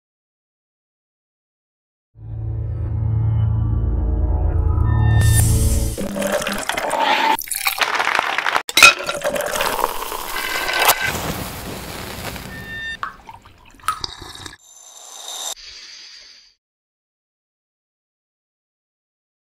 Skinos Mediterranean Cocktails Challenge 2017- Teaser
12 teams, 6 countries, 1 winner!
Get a taste from the grand Final of the Skinos Mediterranean Cocktails Challenge